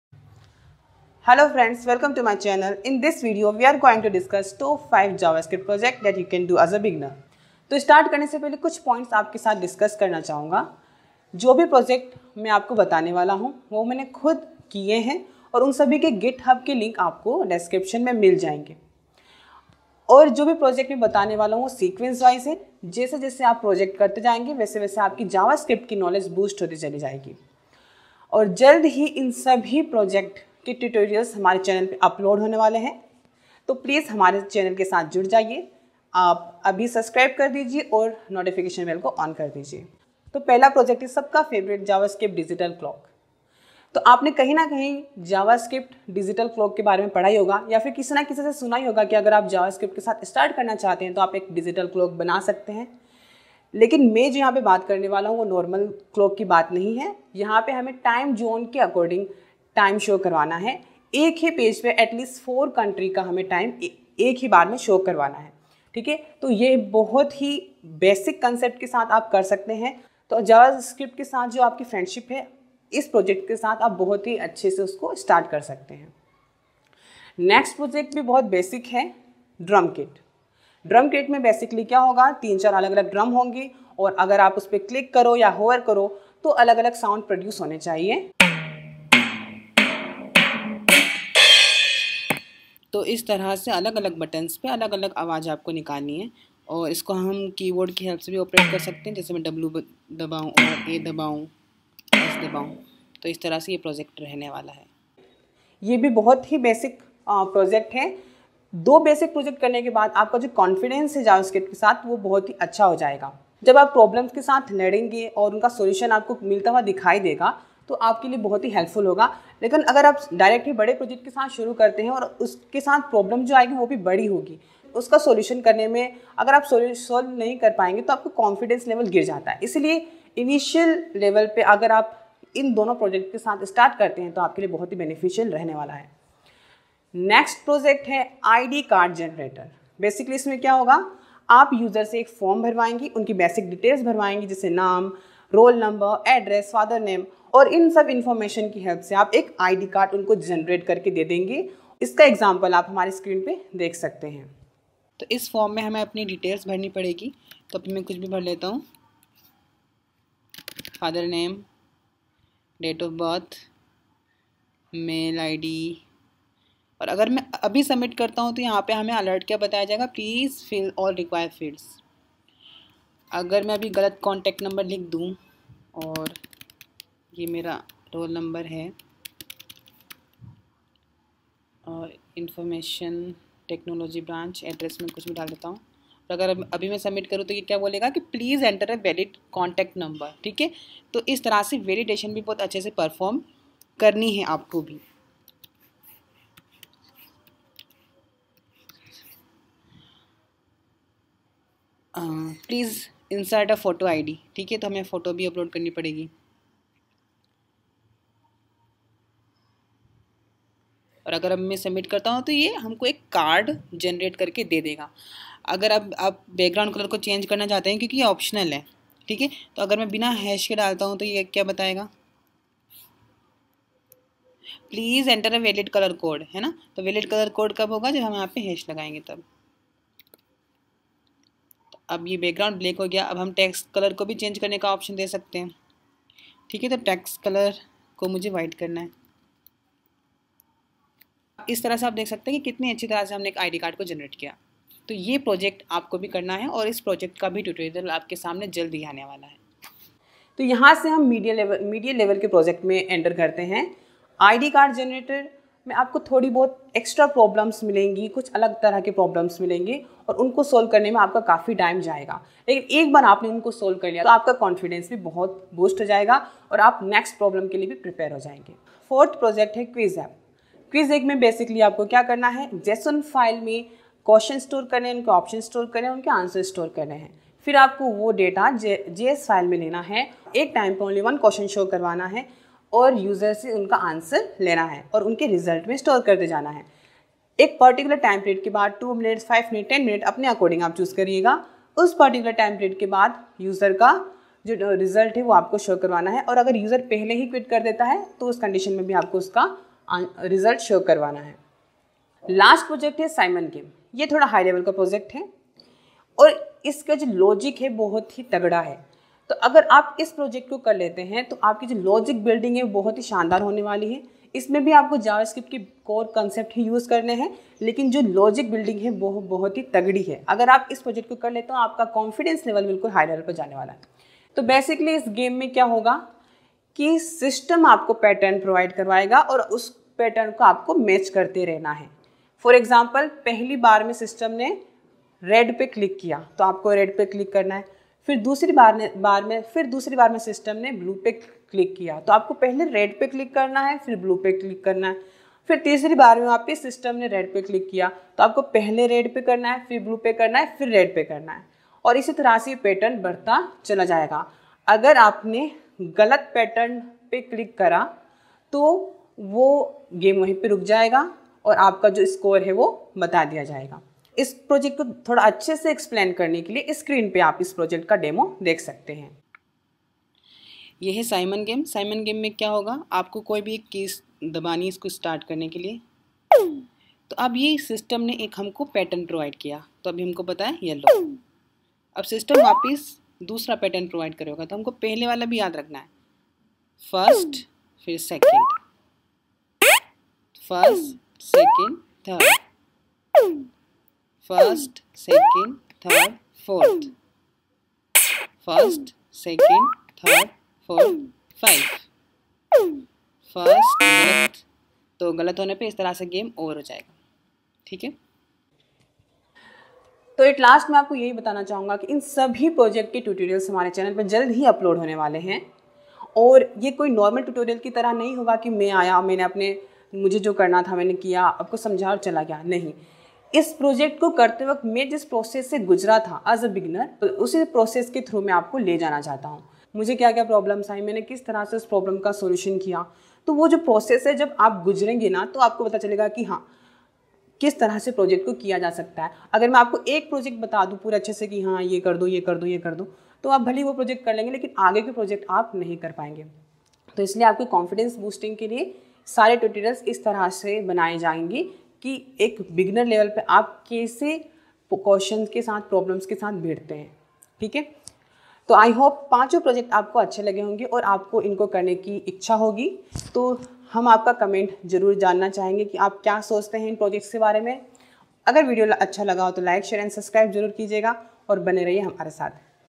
Build 5 Javascript Projects As a Beginner | Javascript Projects For Beginners 2022 | Learn as Intern
Learn as Intern video series...

Projects are a great way to learn any language so that's why In this video, we are discussing 5 javascript project ideas for beginners.
hope these projects will become the way to start a friendship with javascript...

need your feedback and support.

Github repo links:
1. Time by time zone